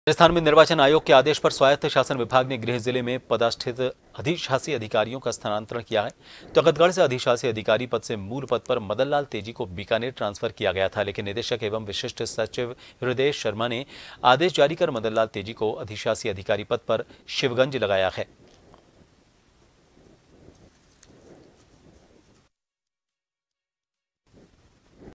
Sumerpur (Raj) News |अधिशासी अधिकारियों के स्थानांतरण, अधिशासी अधिकारी मदन लाल तेजी को लगाया शिवगंज
Watch JAN TV on :
Tata Play DTH : 1185
Airtel DTH: 355
JIO Fiber: 1384

News Credit-MKP